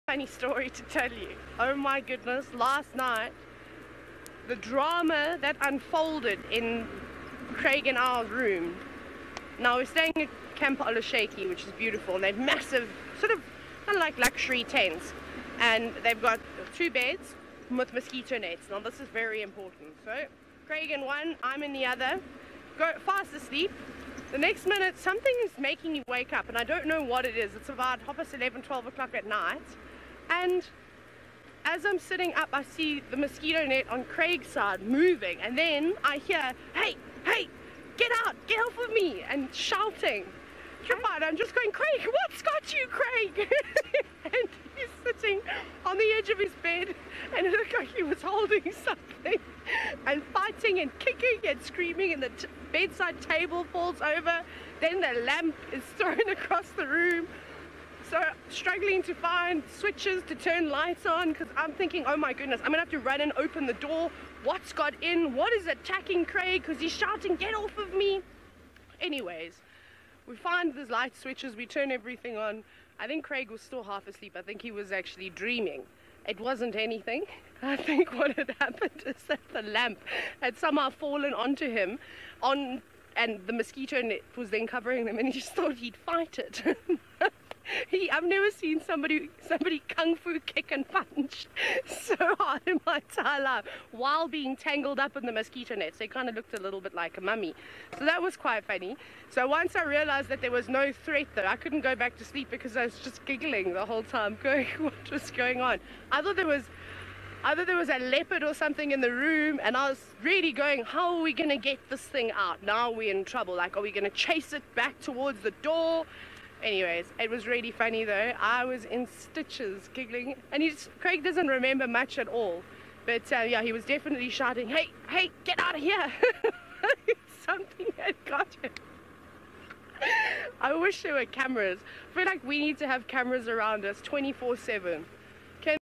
Jan 27 2018 Tayla tells her nighttime story about Craig -Tayla's Giggle is infectious
Jan 27 2018- Sunrise-  Tayla tells her nighttime story about Craig